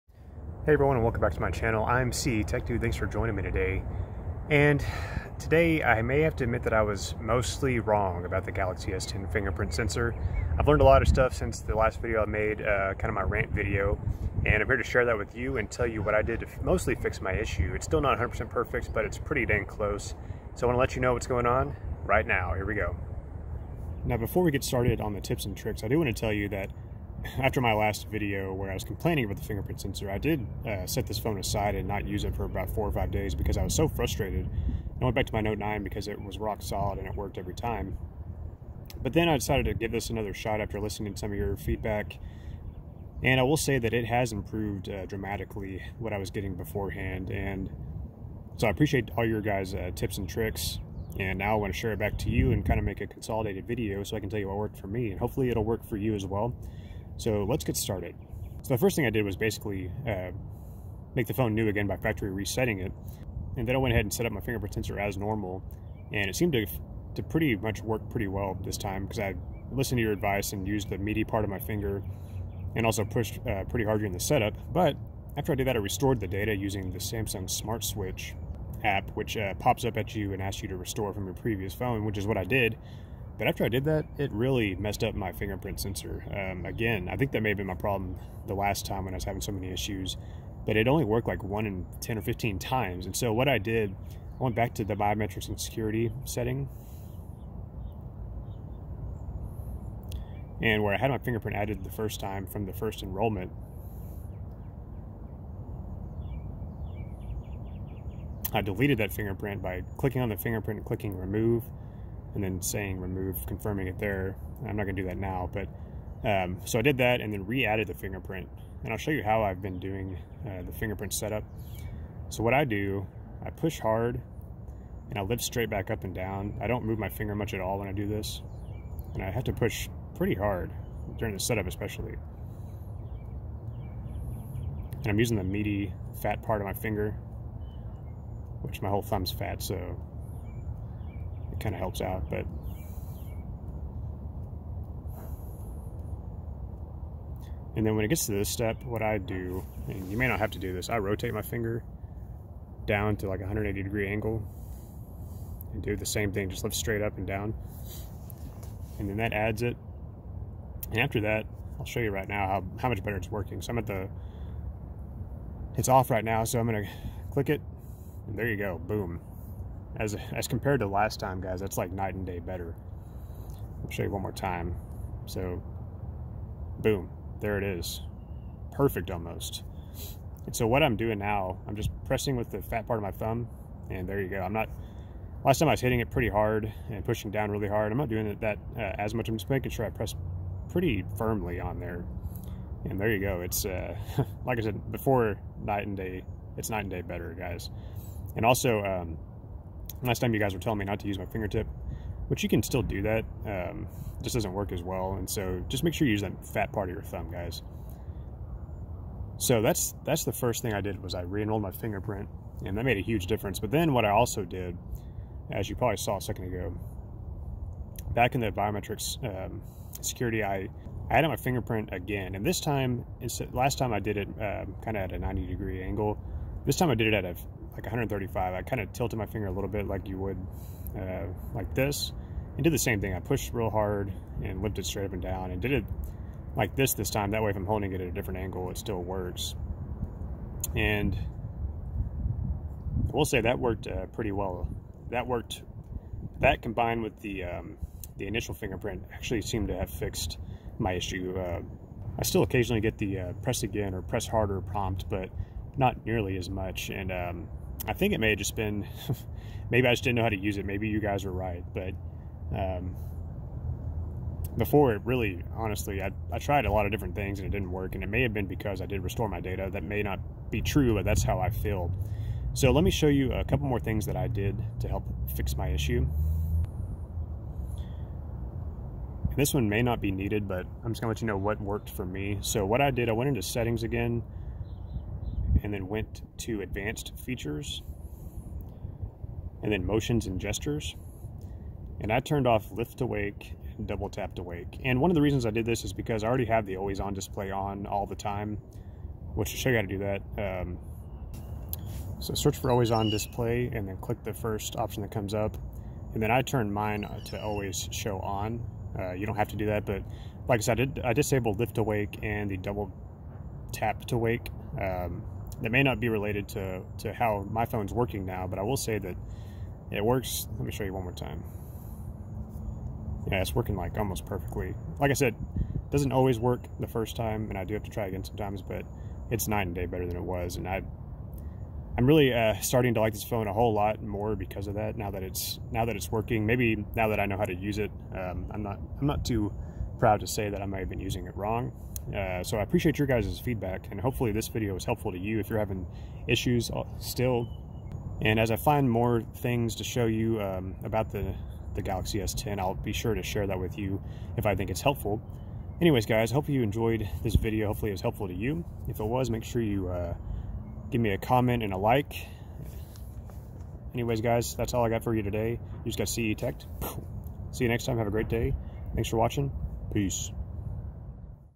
I was (MOSTLY) wrong about the Galaxy S10 fingerprint sensor!
Just wanted to follow up and hopefully give you guys some insight to the Galaxy s10 ultrasonic fingerprint sensor. The sensor was very problematic for me. With your feedback and some trial and error I am now able to use the fingerprint sensor like a normal human being.